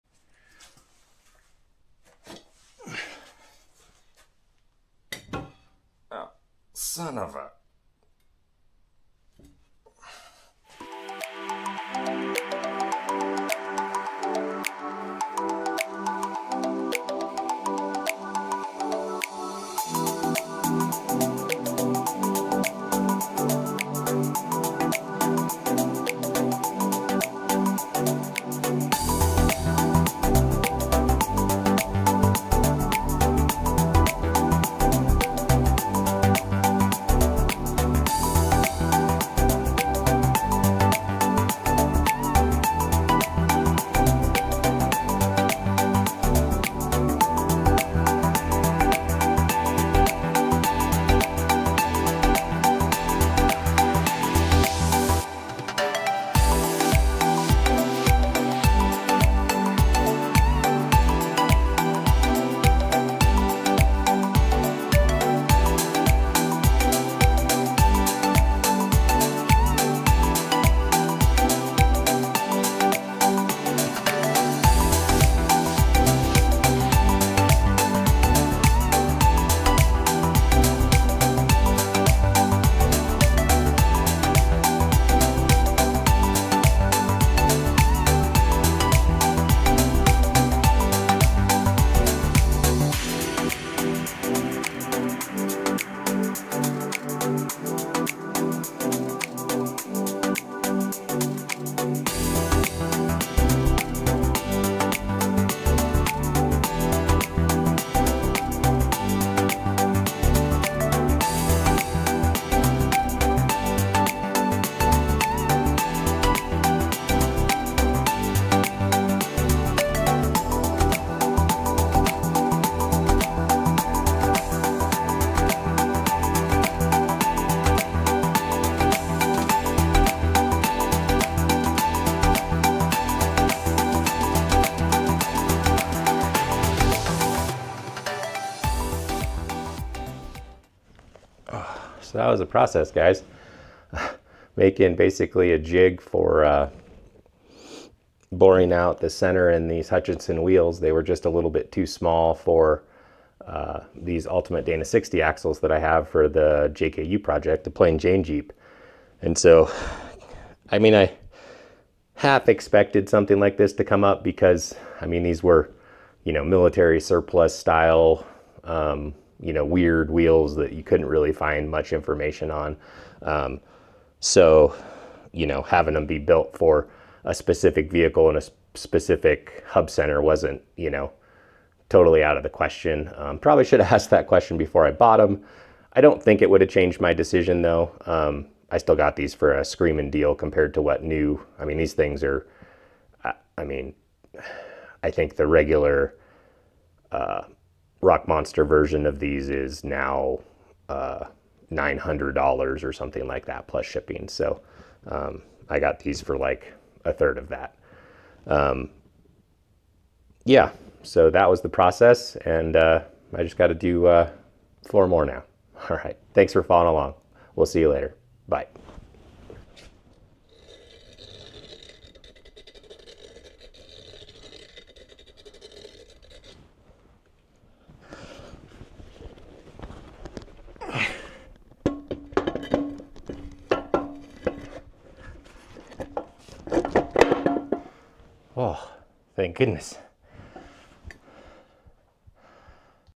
Sidetracked by Boring Wheels
Sometimes, you just have to stop with a project to deal with issues that come up along the way.